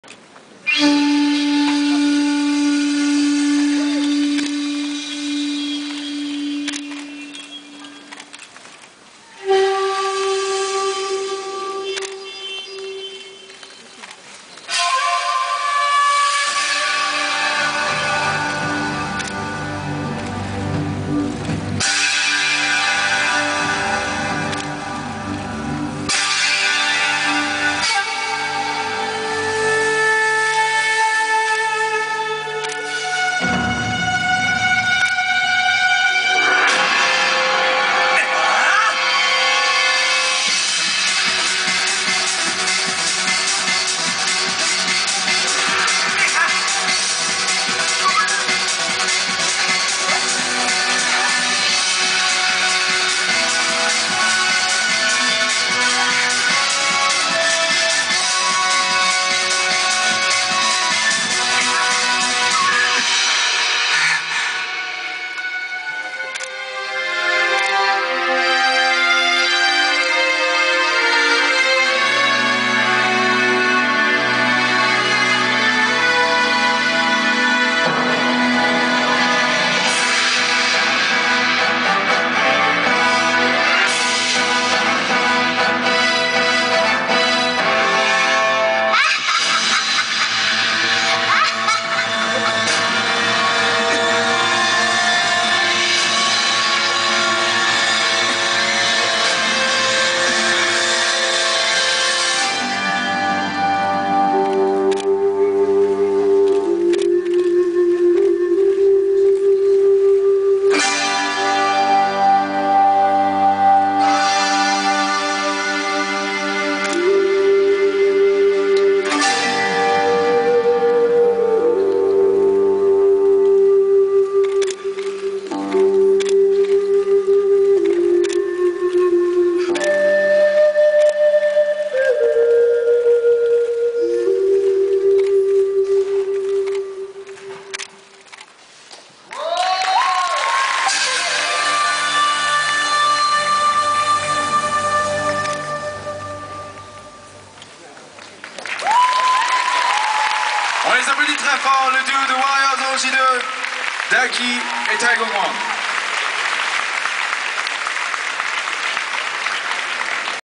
WCS2011 - Japan Expo - French Selection - Warriors Orochi team
Team Warriors Orochi's skit at Japan Expo 2011 during the selection for the French representatives for the World Cosplay Summit 2011.